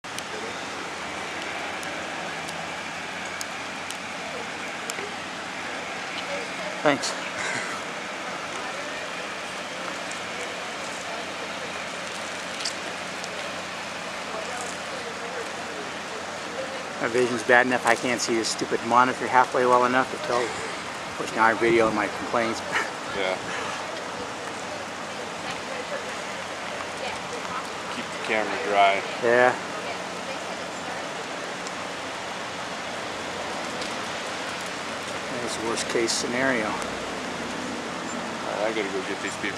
Park Theater Mall Fire
The historic Park Theater Mall burned Monday.